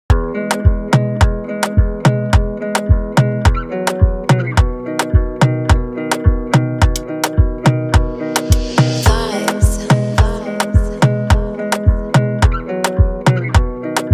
Cute Fawn with Blue Eyes 🦌
These beauties! Enjoying pellets and sunshine. We are beginning to wean down more each week. I just picked up another 300lbs of feed to keep them well fed.

Did you know fawn eat more than goats? They blow through almost a 50lb bag every other day when they are older.
We want them super fat and ready for fall and winter weather, incase they aren't great at foraging and need that extra boost to keep them healthy.